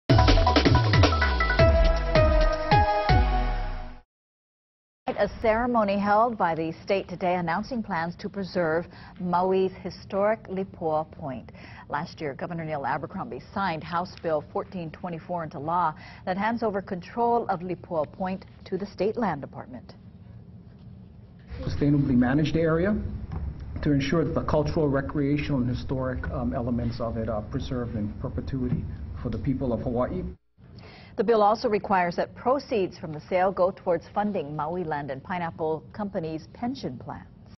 Ceremony marks preservation plans for historic Maui lands
Last year, Gov. Neil Abercrombie signed House Bill 1424 into law, handing over control of Lipoa Point to the State Land Department.